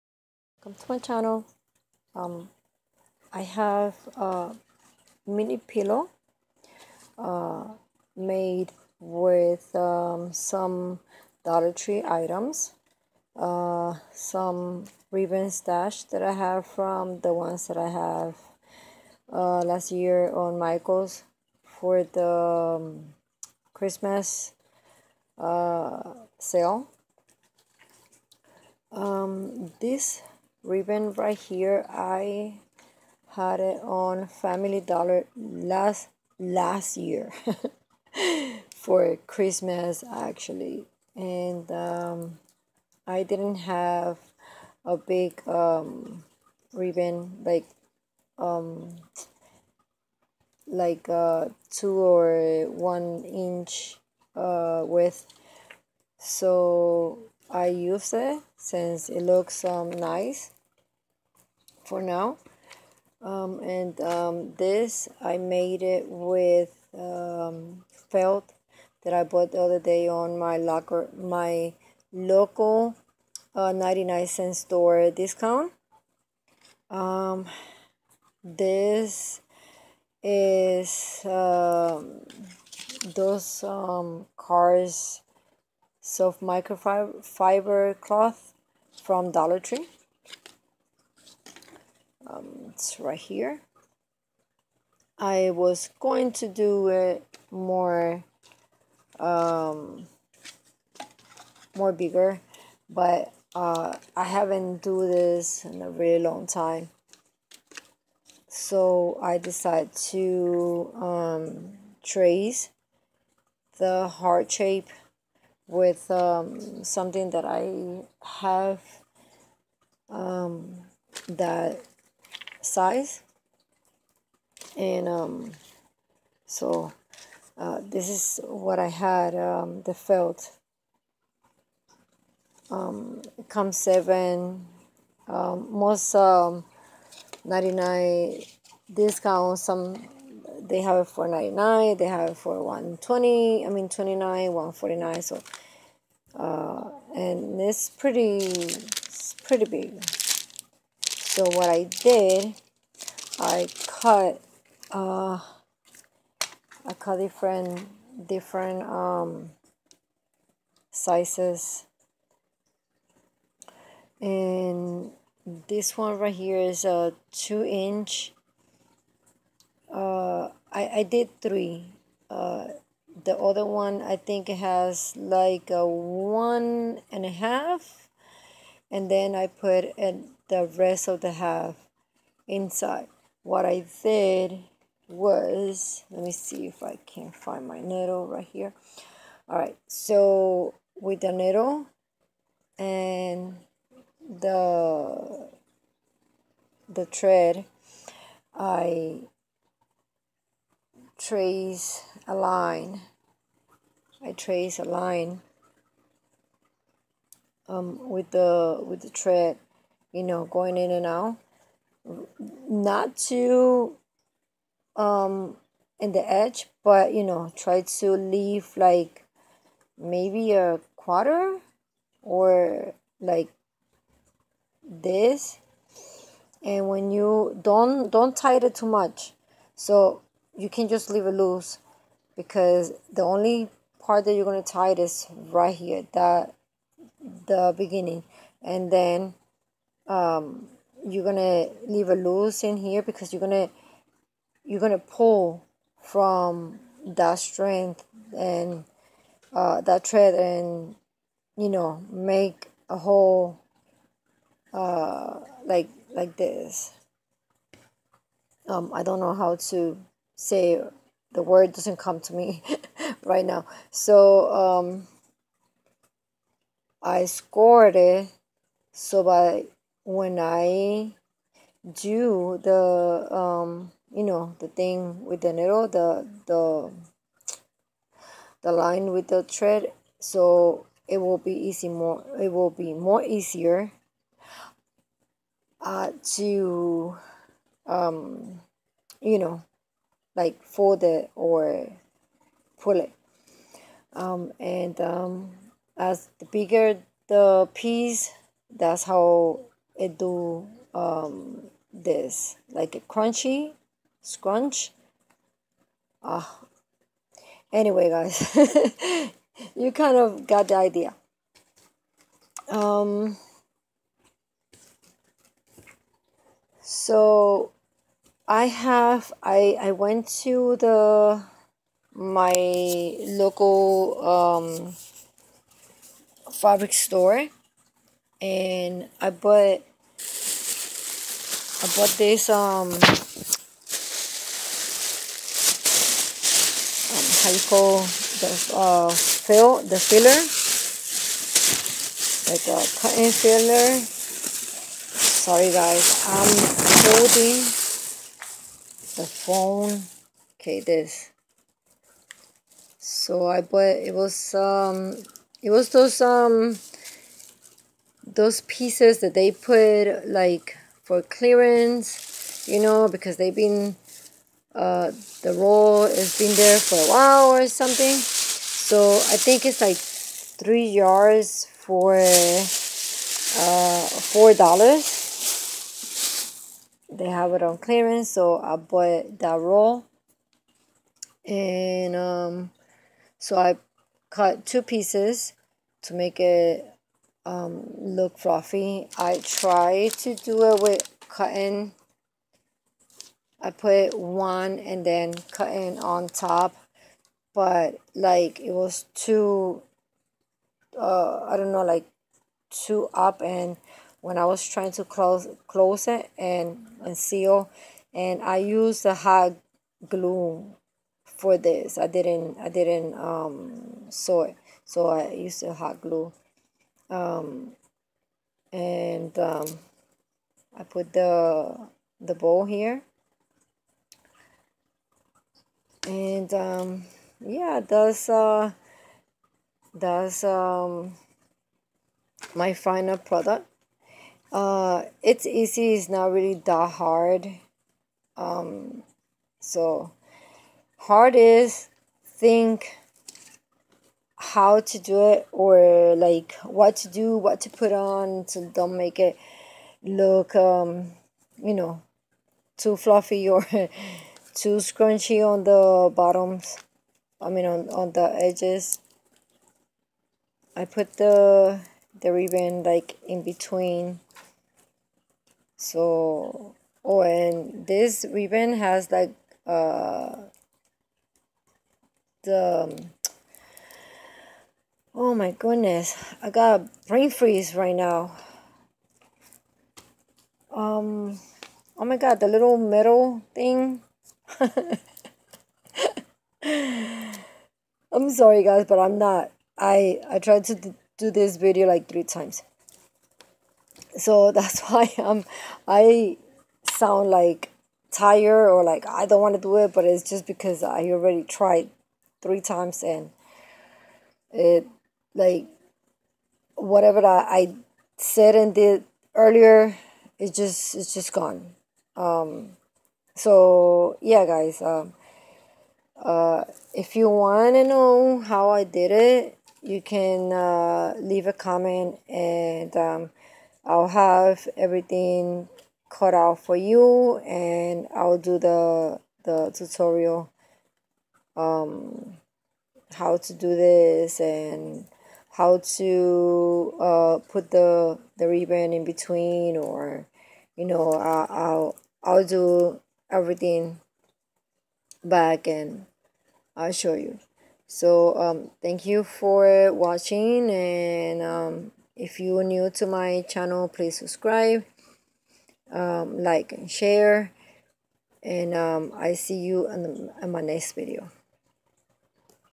Dollar Tree DIY Valentine's Heart 💖 Pillow..
I used DollarTree soft microfiber cloth for the pillow... Michael's last year Christmas ribbon..